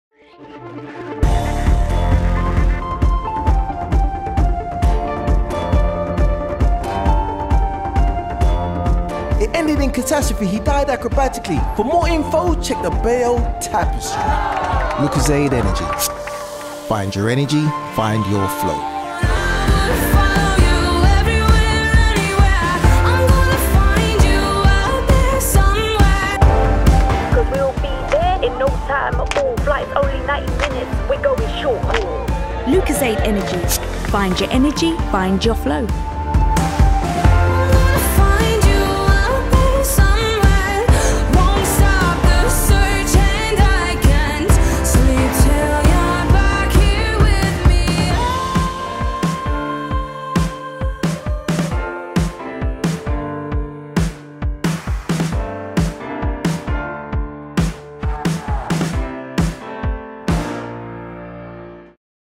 Primesight - Find Your Flow with Lucozade Energy (AE)
Suntory, the new owners of the Lucozade Energy brand, wanted to use its first major campaign to position the drink as relevant for everyday dips in energy levels and Primesight received a brief that targeted ‘everyday strivers’ on the move. Given that this was such a high-profile campaign for the brand, we realised that we’d need to do something extra special – something that had never been done before. Together with MediaCom and GTN UK we faced up to and surmounted considerable technical challenges to launch a campaign that synchronized radio traffic bulletins with our digital roadside screens for a unique and highly effective campaign.